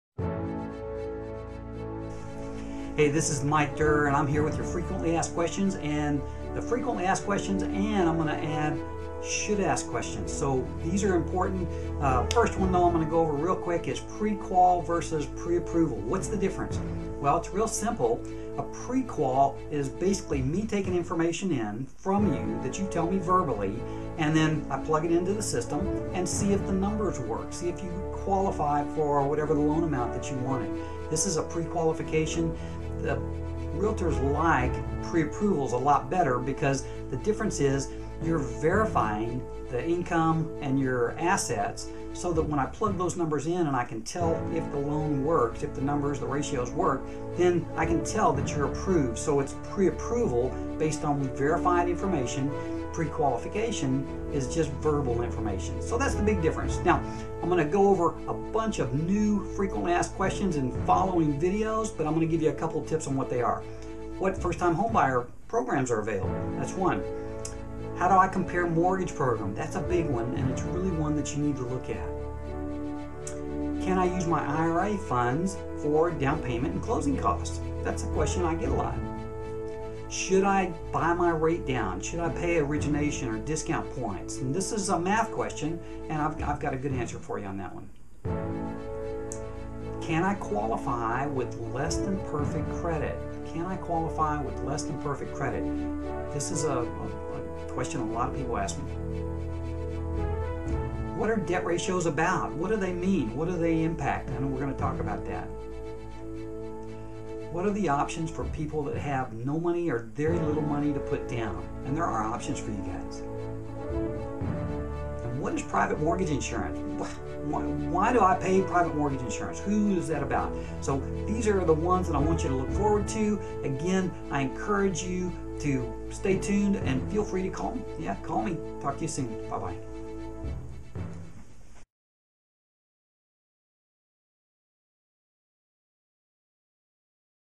Houston Mortgage FAQ
Mike Durr (Kingwood Mortgage Guy) answers the most frequently asked questions home buyers ask, and answers the questions the home buyers should ask. This is part one in a series of videos designed to help home buyers understand the confusing mortgage process.

After working in the industry for 15 years and completing thousands of transactions, I got together with my team and we went over the questions that most buyers ask us consistently.  Then we discussed what should the home buyers have asked, but just didn't know to ask.

We came up with an initial list of 10 of the big questions that should go a long way towards giving the home buyer a better idea as to what they need to know when buying a residential property.

The whole idea of the video series is to give home buyers a great chance to save money when completing one of the biggest financial trasactions of their life.

hl=en